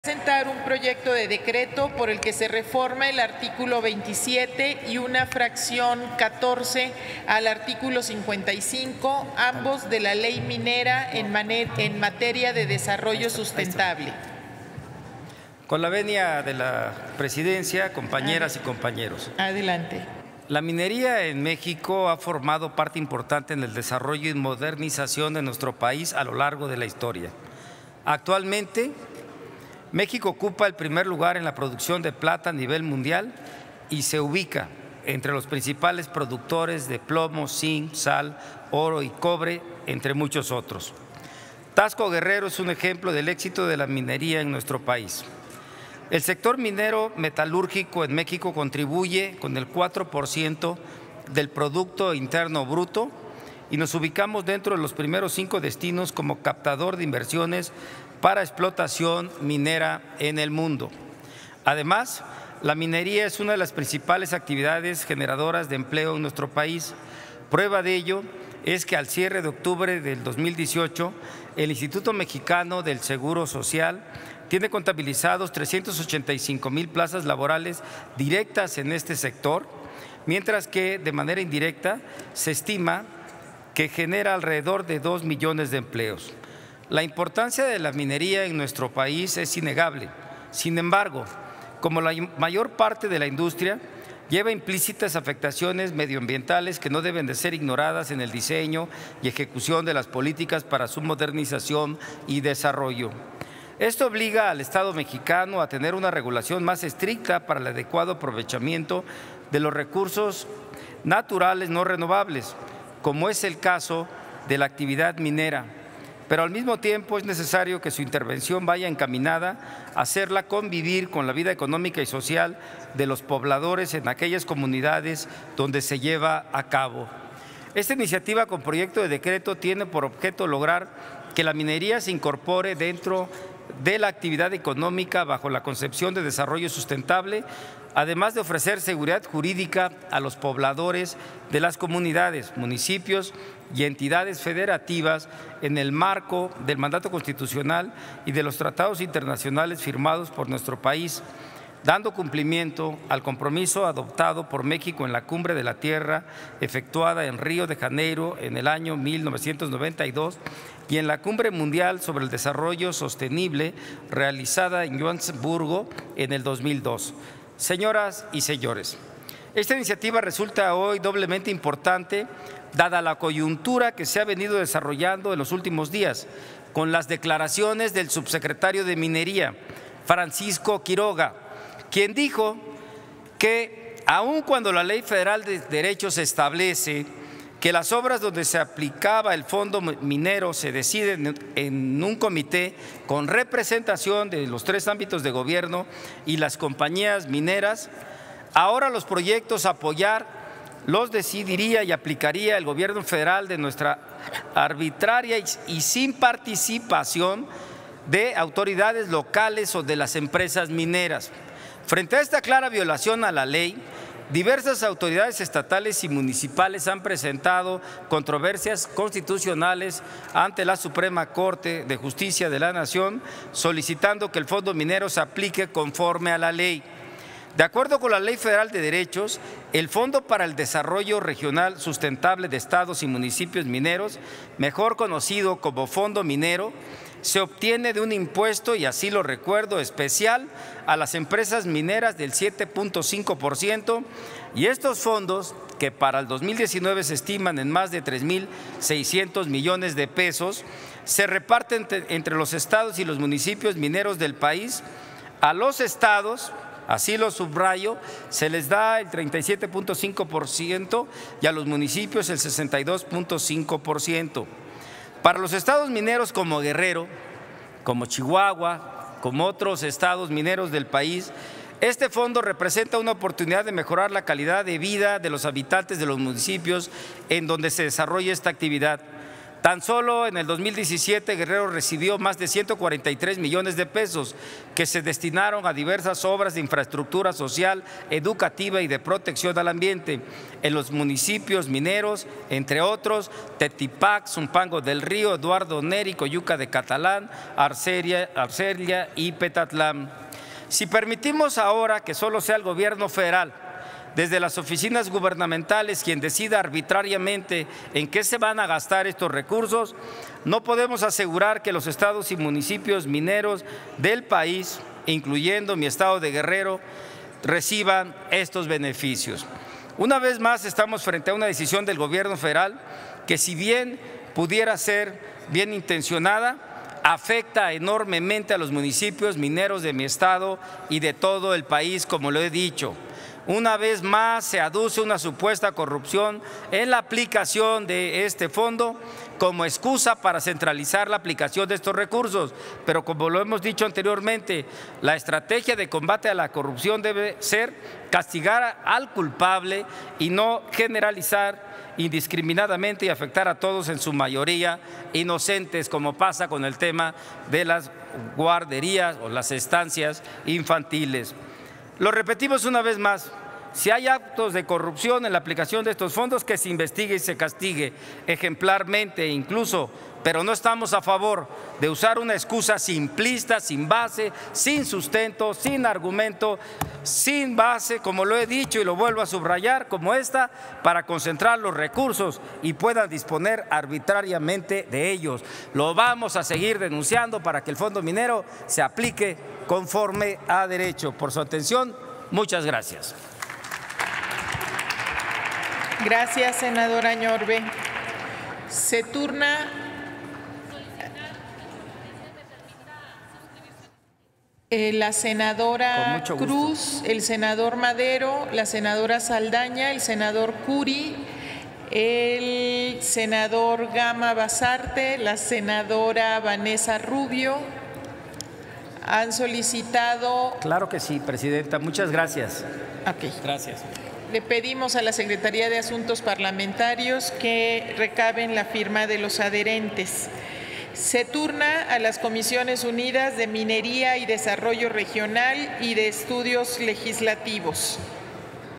Sen. Añorve: Que Fondo Minero se destine a mejorar calidad de vida de comunidades mineras
El senador Manuel Añorve, del Grupo Parlamentario del PRI, presenta iniciativa para adicionar la Ley Minera.

Señala que el Fondo para el Desarrollo Regional Sustentable de Estados y Municipios Mineros se obtiene del impuesto especial a empresas mineras y se reparte entre los estados y municipios donde se lleva a cabo esta actividad económica. Pide que el Gobierno Federal no decida arbitrariamente en qué se gastarán los recursos de este Fondo.